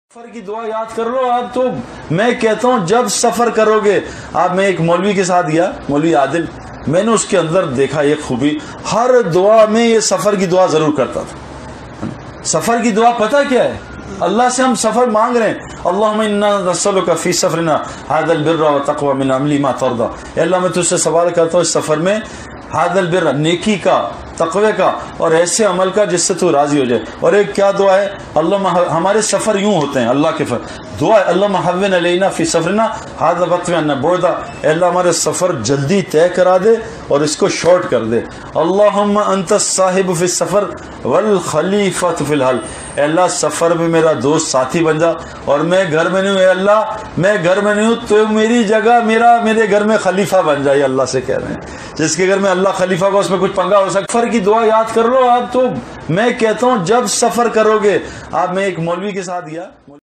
🎤Saeed Anwar Bayan🔛Safar Ki Dua
Safar ki dua
Allahumma "inna nas'aluka fi safarina hadhal-birra wat-taqwa, wa-minal-'amalima tarda,
Allahumma hawwin 'alayna safarana hadha watwi 'anna bu'dahu, Allahumma "Antas-sahibu fis-safari, wal-khalifatu fil-"ahli, Allahumma "inni "a'udhu bika min wa'tha"is-safari, wa ka'abanl-manzari, wa su"il-munqalabi fil-maliwal"ahli.